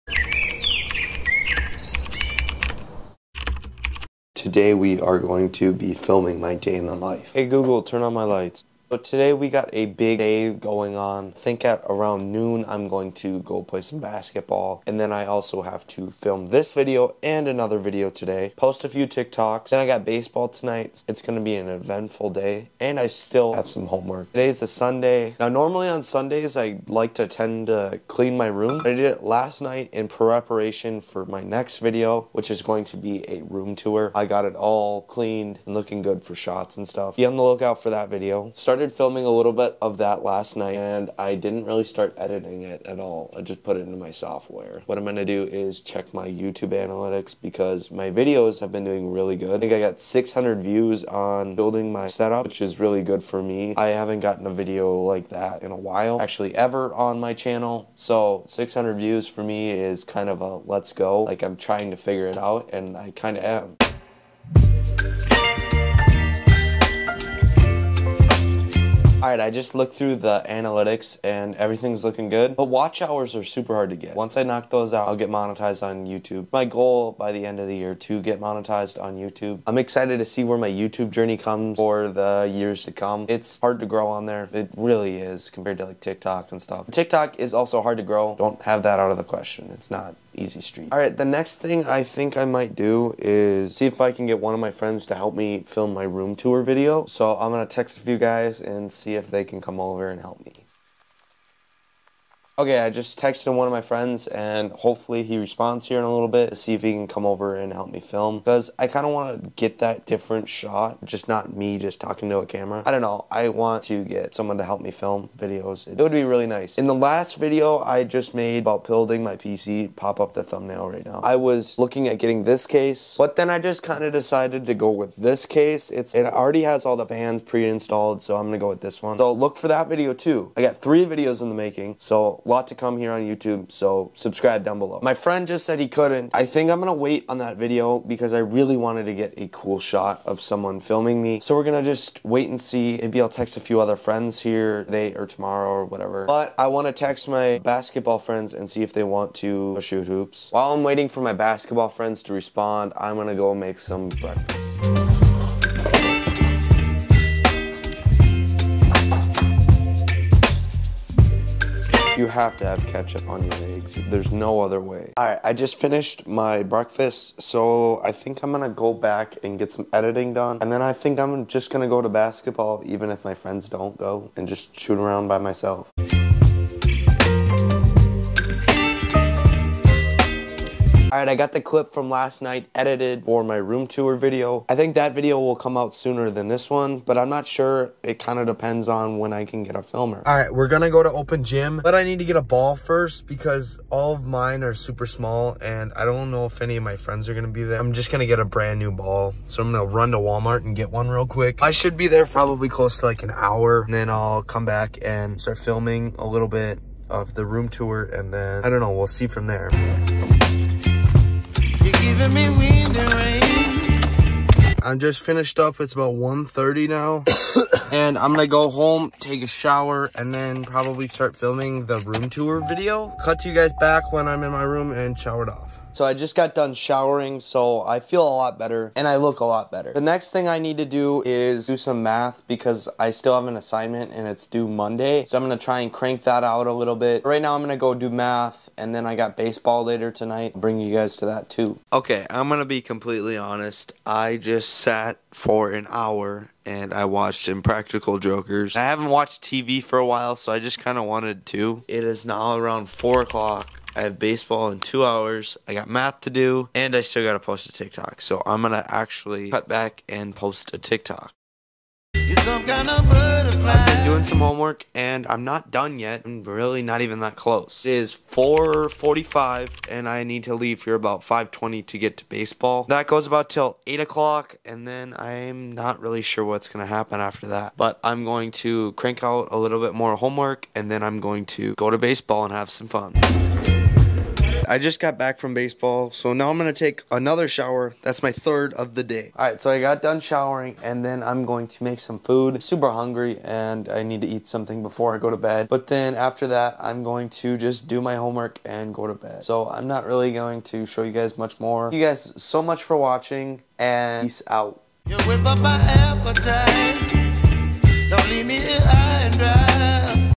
Day In The Life Of A 16 Year Old Content Creator
I love you Guys I've been having some issues if you didn't know with my phone being stolen I'm going to keep grinding for you guy I truly love you guys

Thumbnail idea @masoncargile